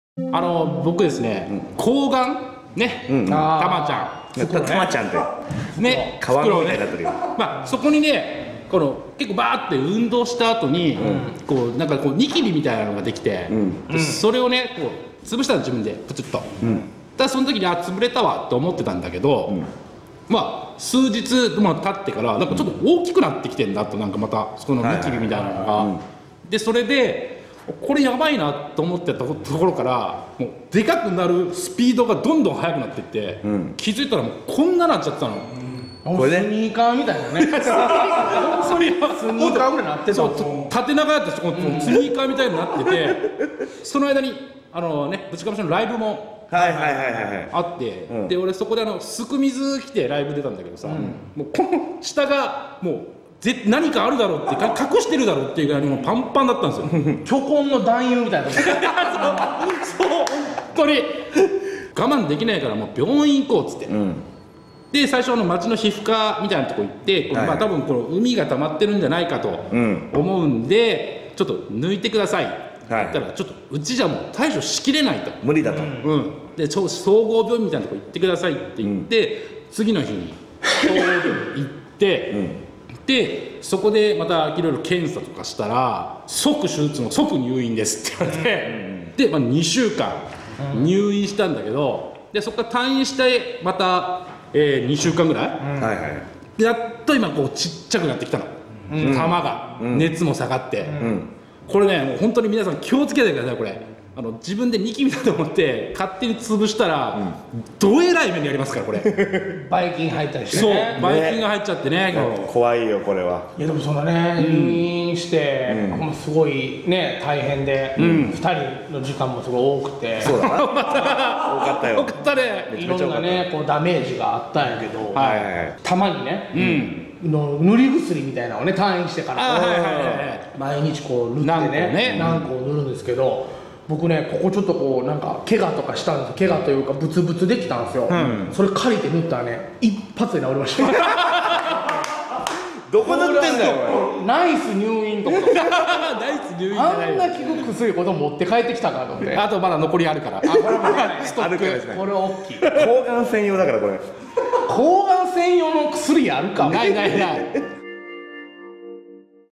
○○にできたニキビをつぶす→即手術＆入院の事態に！ よしログ
2019年08月22日（木）ブチかましのよしログ。YouTuber芸人のブチかまし・たっつぁんは、男の大事な部分に出来たニキビを何気なくつぶしたという。すると、見る見るうちに患部が腫れ上がり……。

【出演】
ブチかまし